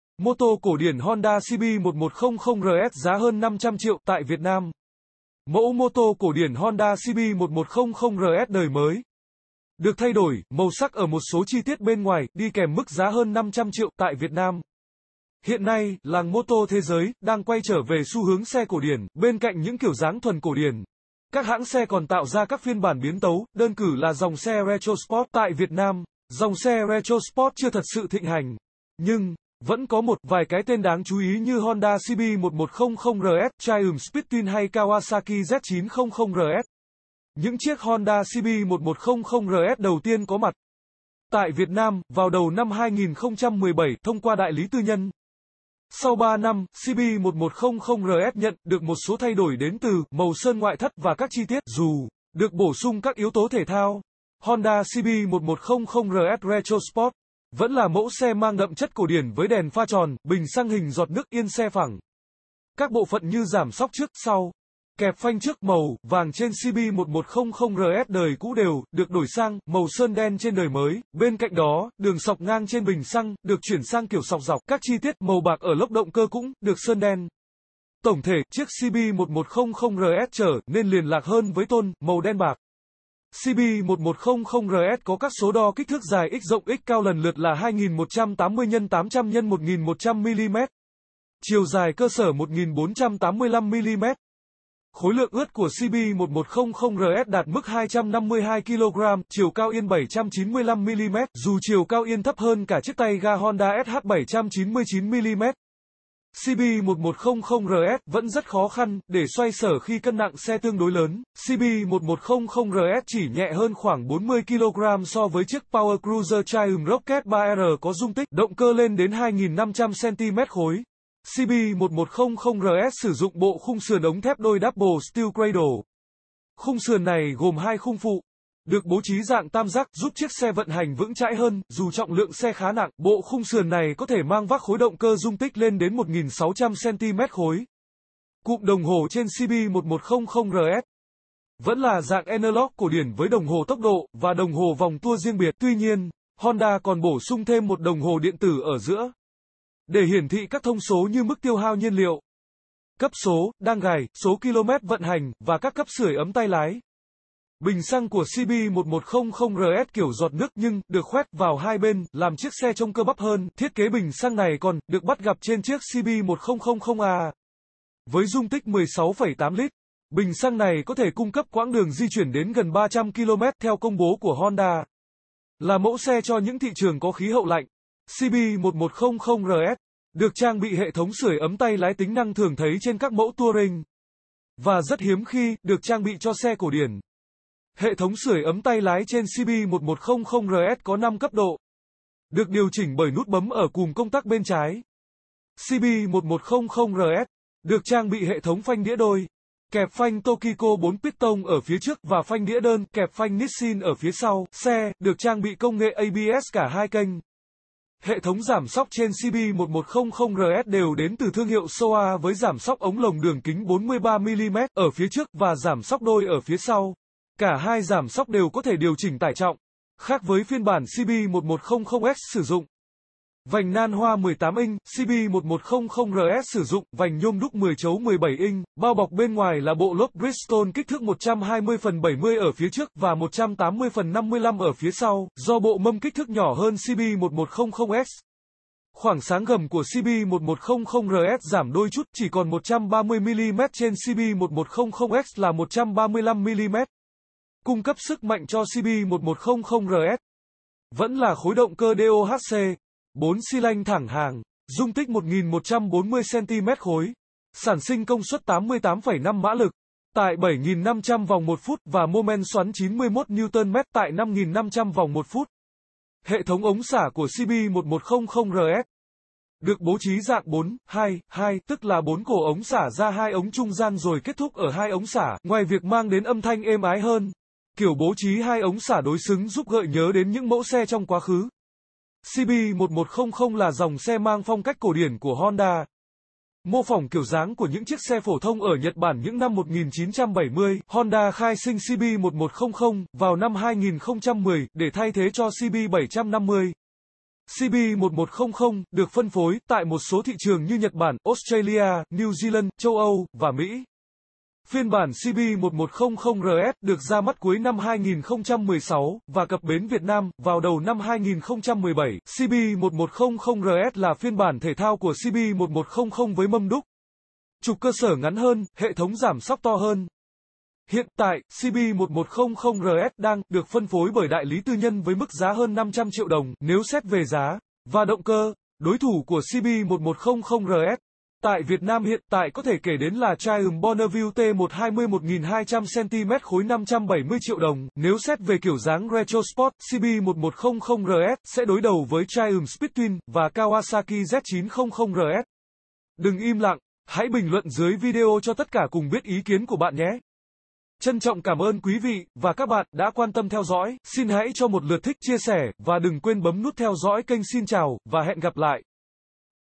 Môtô cổ điển Honda CB1100 RS giá hơn 500 triệu tại VN
Tải Game Android hay Miễn Phí Đăng kí kênh xem tin tức mới nhất Mẫu môtô cổ điển Honda CB1100 RS đời mới được thay đổi màu sắc ở một số chi tiết bên ngoài, đi kèm mức giá hơn 500 triệu tại Việt Nam.Hiện nay, làng môtô thế giới đang quay trở về xu hướng xe cổ điển
Bên cạnh những kiểu dáng thuần cổ điển, các hãng xe còn tạo ra các phiên bản biến tấu, đơn cử là dòng xe retro-sport
Tại Việt Nam, dòng xe retro-sport chưa thật sự thịnh hành nhưng vẫn có một vài cái tên đáng chú ý như Honda CB1100 RS, Triumph Speed Twin hay Kawasaki Z900 RS.Những chiếc Honda CB1100 RS đầu tiên có mặt tại Việt Nam vào đầu năm 2017, thông qua đại lý tư nhân
Sau 3 năm, CB1100 RS nhận được một số thay đổi đến từ màu sơn ngoại thất và các chi tiết
Dù được bổ sung các yếu tố thể thao, Honda CB1100 RS (retro-sport) vẫn là mẫu xe mang đậm chất cổ điển với đèn pha tròn, bình xăng hình giọt nước, yên xe phẳng.Các bộ phận như giảm xóc trước/sau, kẹp phanh trước màu vàng trên CB1100 RS đời cũ đều được đổi sang màu sơn đen trên đời mới
Bên cạnh đó, đường sọc ngang trên bình xăng được chuyển sang kiểu sọc dọc
Các chi tiết màu bạc ở lốc động cơ cũng được sơn đen
Tổng thể, chiếc CB1100 RS trở nên liền lạc hơn với tone màu đen bạc.CB1100 RS có các số đo kích thước dài x rộng x cao lần lượt là 2.180 x 800 x 1.100 (mm), chiều dài cơ sở 1.485 mm
Khối lượng ướt của CB1100 RS đạt mức 252 kg, chiều cao yên 795 mm
Dù chiều cao yên thấp hơn cả chiếc tay ga Honda SH (799 mm), CB1100 RS vẫn rất khó khăn để xoay xở khi cân nặng xe tương đối lớn
CB1100 RS chỉ nhẹ hơn khoảng 40 kg so với chiếc power cruiser Triumph Rocket 3 R có dung tích động cơ lên đến 2.500 cc.CB1100 RS sử dụng bộ khung sườn ống thép đôi (double steel cradle)
Khung sườn này gồm 2 khung phụ được bố trí dạng tam giác, giúp chiếc xe vận hành vững chãi hơn dù trọng lượng xe khá nặng
Bộ khung sườn này có thể mang vác khối động cơ dung tích lên đến 1.600 cc.Cụm đồng hồ trên CB1100 RS vẫn là dạng analog cổ điển với đồng hồ tốc độ và đồng hồ vòng tua riêng biệt
Tuy nhiên, Honda còn bổ sung thêm một đồng hồ điện tử ở giữa để hiển thị các thông số như mức tiêu hao nhiên liệu, cấp số đang gài, số km vận hành và các cấp sưởi ấm tay lái.Bình xăng của CB1100 RS kiểu giọt nước nhưng được khoét vào 2 bên, làm chiếc xe trông cơ bắp hơn
Thiết kế bình xăng này còn được bắt gặp trên chiếc CB1000R
Với dung tích 16,8 lít, bình xăng này có thể cung cấp quãng đường di chuyển đến gần 300 km, theo công bố của Honda.Là mẫu xe cho những thị trường có khí hậu lạnh, CB1100 RS được trang bị hệ thống sưởi ấm tay lái - tính năng thường thấy trên các mẫu touring và rất hiếm khi được trang bị cho xe cổ điển
Hệ thống sưởi ấm tay lái trên CB1100 RS có 5 cấp độ, được điều chỉnh bởi nút bấm ở cùm công tắc bên trái.CB1100 RS được trang bị hệ thống phanh đĩa đôi, kẹp phanh Tokico 4 piston ở phía trước và phanh đĩa đơn, kẹp phanh Nissin ở phía sau
Xe được trang bị công nghệ ABS cả 2 kênh.Hệ thống giảm xóc trên CB1100 RS đều đến từ thương hiệu Showa với giảm xóc ống lồng đường kính 43 mm ở phía trước và giảm xóc đôi ở phía sau
Cả 2 giảm xóc đều có thể điều chỉnh tải trọng.Khác với phiên bản CB1100 EX sử dụng vành nan hoa 18 inch, CB1100 RS sử dụng vành nhôm đúc 10 chấu 17 inch
Bao bọc bên ngoài là bộ lốp Bridgestone kích thước 120/70 ở phía trước và 180/55 ở phía sau
Do bộ mâm kích thước nhỏ hơn CB1100 EX, khoảng sáng gầm của CB1100 RS giảm đôi chút, chỉ còn 130 mm (trên CB1100 EX là 135 mm).Cung cấp sức mạnh cho CB1100 RS vẫn là khối động cơ DOHC, 4 xy-lanh thẳng hàng, dung tích 1.140 cc, sản sinh công suất 88,5 mã lực tại 7.500 vòng/phút và mô-men xoắn 91 Nm tại 5.500 vòng/phút.Hệ thống ống xả của CB1100 RS được bố trí dạng 4-2-2, tức là 4 cổ ống xả ra 2 ống trung gian rồi kết thúc ở 2 ống xả
Ngoài việc mang đến âm thanh êm ái hơn, kiểu bố trí 2 ống xả đối xứng giúp gợi nhớ đến những mẫu xe trong quá khứ.CB1100 là dòng xe mang phong cách cổ điển của Honda, mô phỏng kiểu dáng của những chiếc xe phổ thông ở Nhật Bản những năm 1970
Honda khai sinh CB1100 vào năm 2010 để thay thế cho CB750
CB1100 được phân phối tại một số thị trường như Nhật Bản, Australia, New Zealand, châu Âu và Mỹ.Phiên bản CB1100 RS được ra mắt cuối năm 2016 và cập bến Việt Nam vào đầu năm 2017
CB1100 RS là phiên bản thể thao của CB1100 với mâm đúc, trục cơ sở ngắn hơn, hệ thống giảm xóc to hơn…
Hiện tại, CB1100 RS đang được phân phối bởi đại lý tư nhân với mức giá hơn 500 triệu đồng
Nếu xét về giá và động cơ, đối thủ của CB1100 RS tại Việt Nam hiện tại có thể kể đến là Triumph Bonneville T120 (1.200 cc - 570 triệu đồng)
Nếu xét về kiểu dáng retro-sport, CB1100 RS sẽ đối đầu với Triumph Speed Twin và Kawasaki Z900 RS.